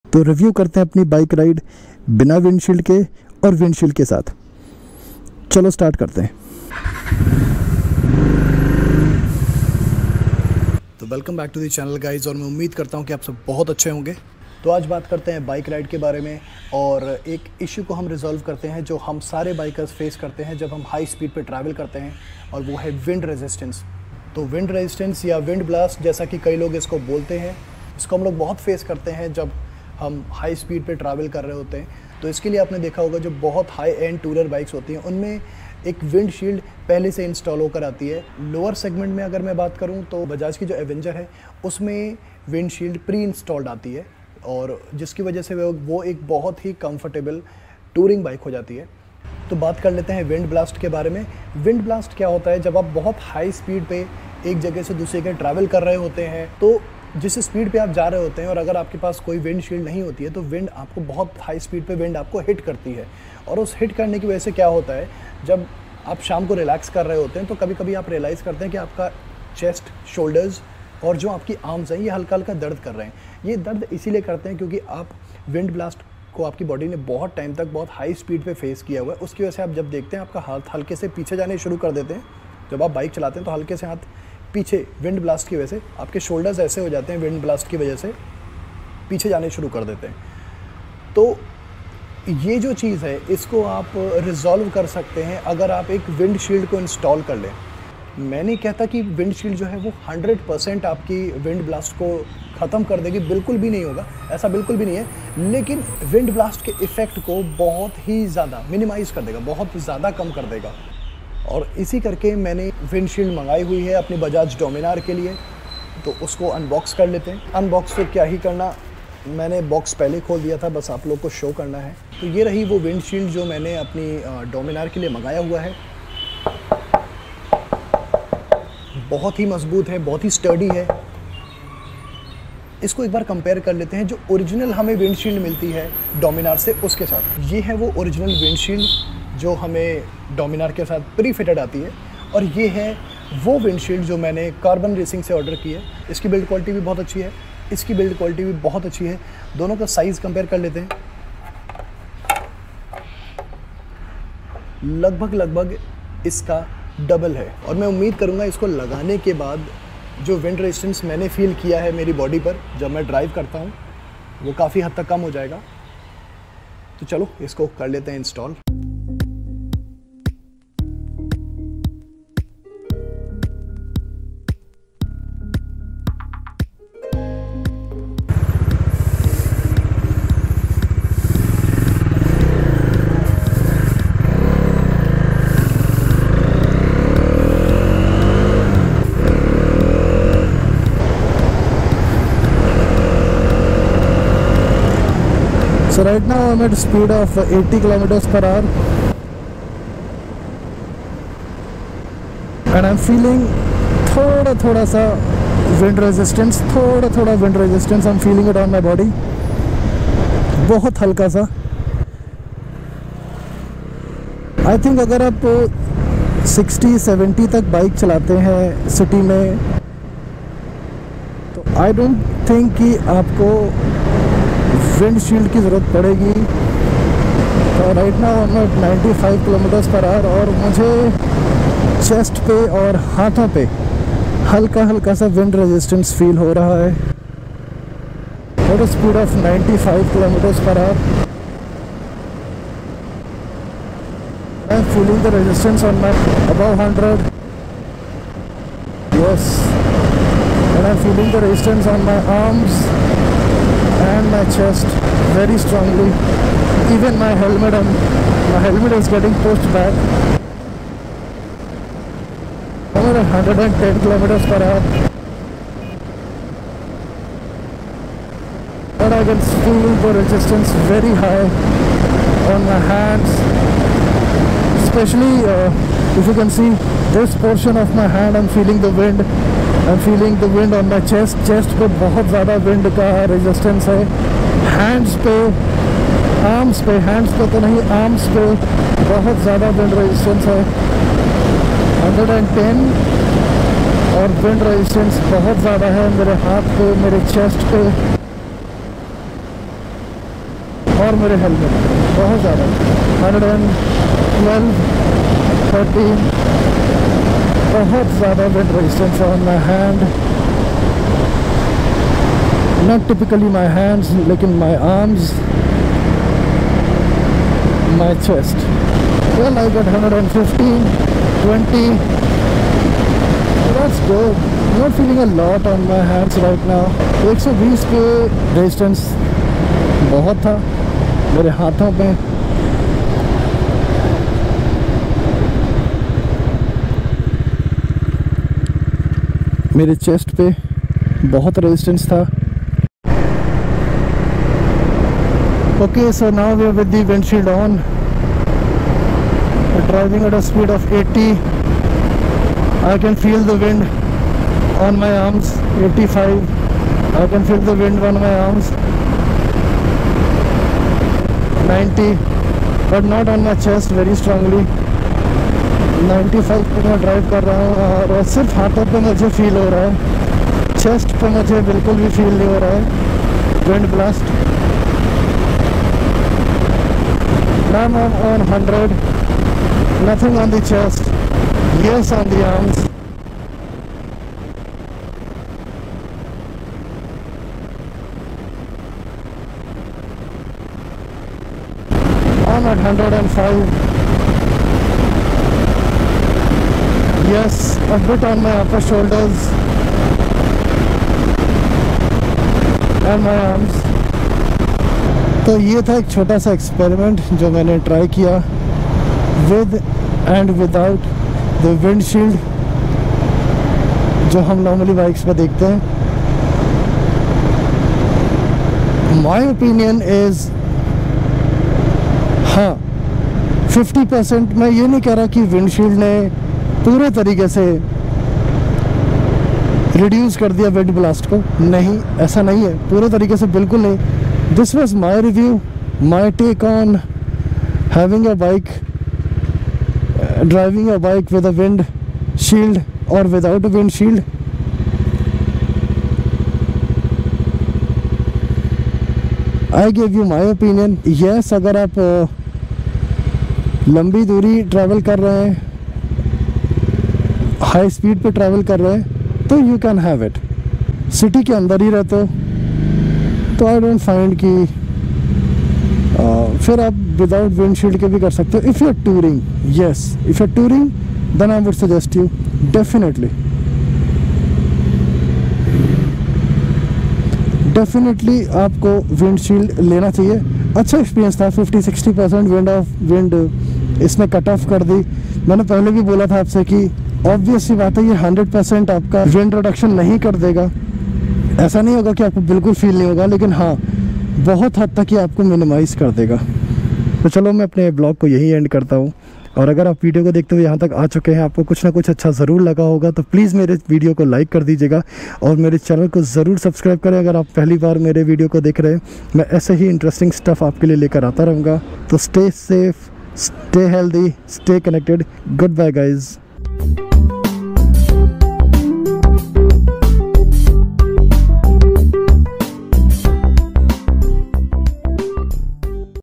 Motorcycle Windshield - Do we need it?
The difference between a motorcycle with a windshield vs a motorcycle without a windshield. I was not aware of this when i purchased the bike, but when i started touring on my bike, i realized the need of it.